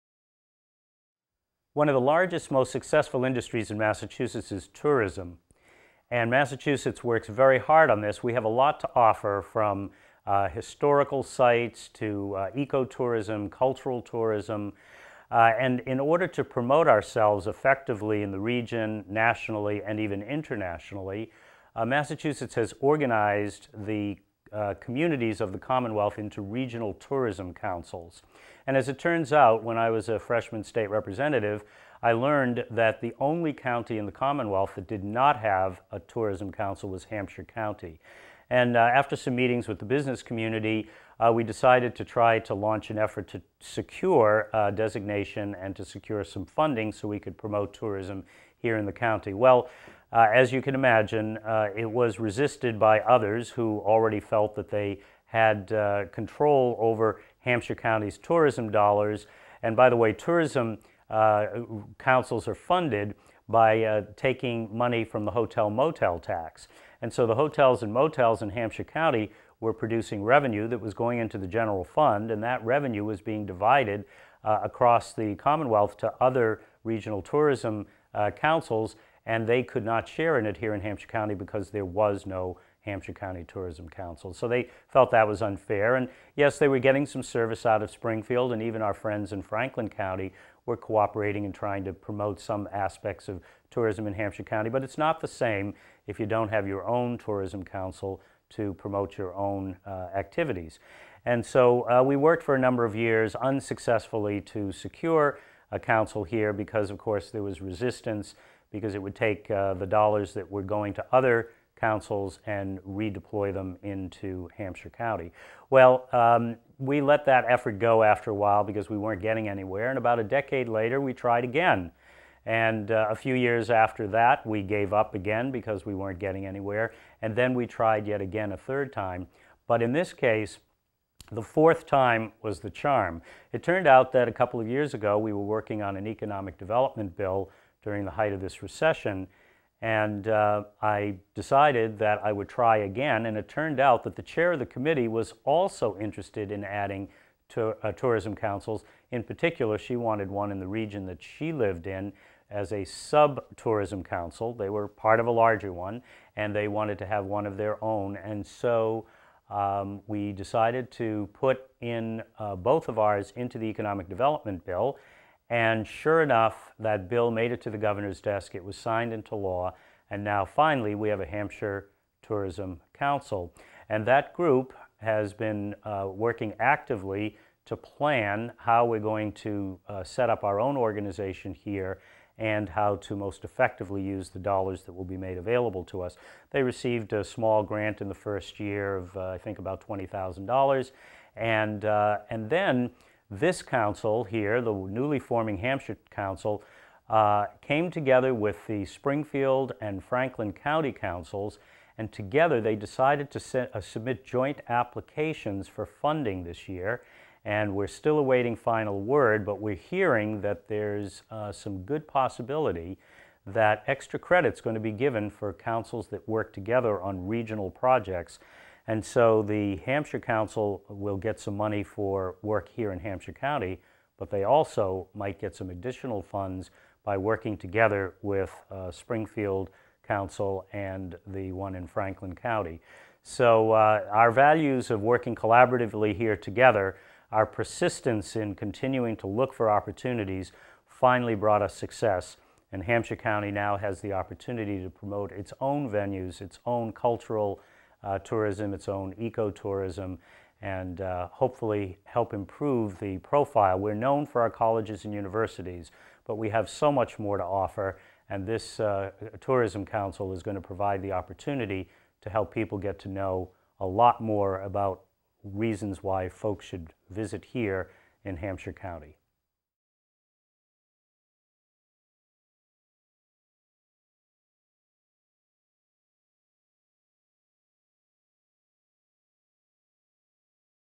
Tourism Council
Senator Rosenberg successfully created a Hampshire County Regional Tourism Council. The regional tourism council will market the attractions of the area, and will help to generate tourism and recreational activities.